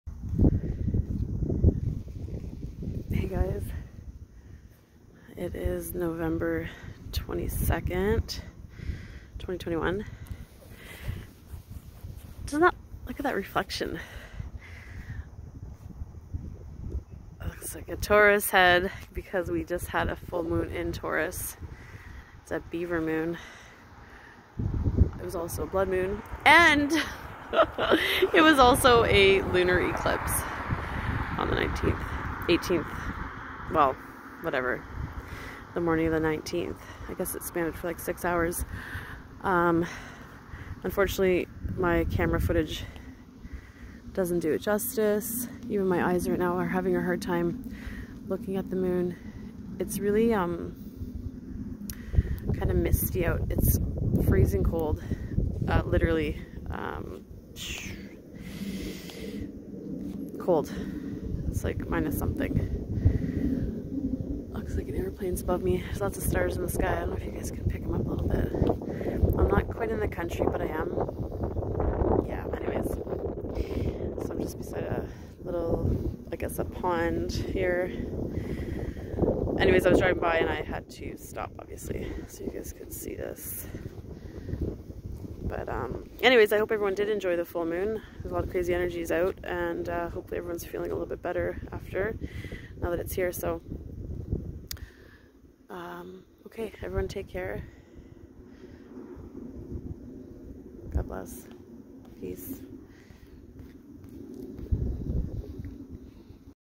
Once Upon A Beaver Moon...🦫🌕
This only happens once every thousamd years.. this of course is days after but still enjoying the energy! Wasn't quite a full lunar  eclipse, 97% but close enough for me! 😆 🐃🩸🦫🌕🌑☀️🌌⭐ God bless everyone❣